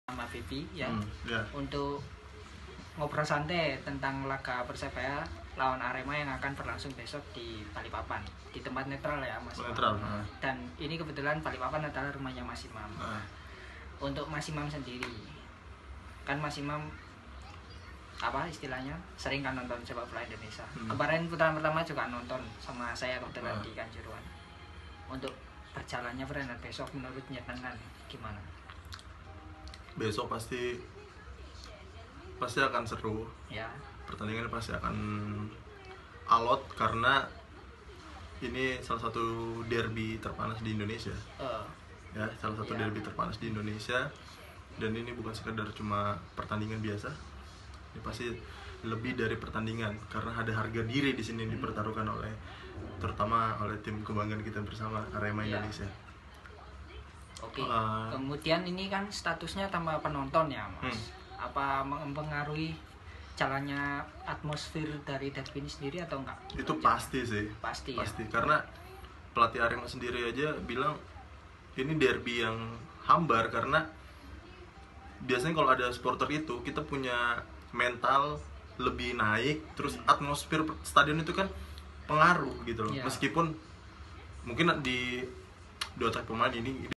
Ngobrol Santai Jelang Persebaya vs Arema Part 1.
Kali ini saya akan ngobrol santai dengan Mas Imam Habibie.

Jangan Lupa ada Part 2 ya. Heheheee